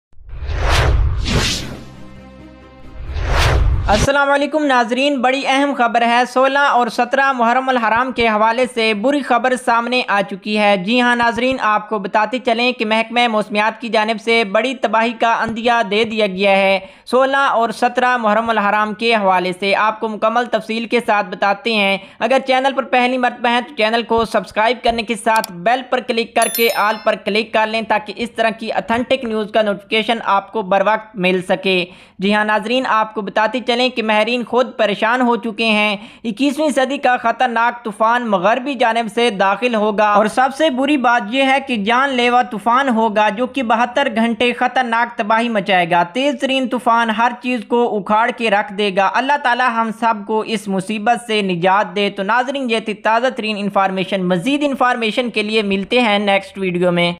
اللہ اکبر 16اور 17 محرم الحرام کے دن اکیسویں صدی کا خطر ناک طوفان // بڑی تباہی ہر طرف لاشیں ملیں گی
and other latest update